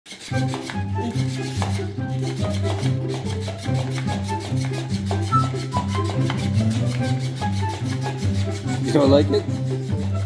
Cute American Bully Reacts To Flappy Fish Toy 🤣
My Pocket American Bully puppy got a new floppy fish toy and his cute reaction to it made me laugh. Very unsure about what to do with this new dog toy that moves around on its own.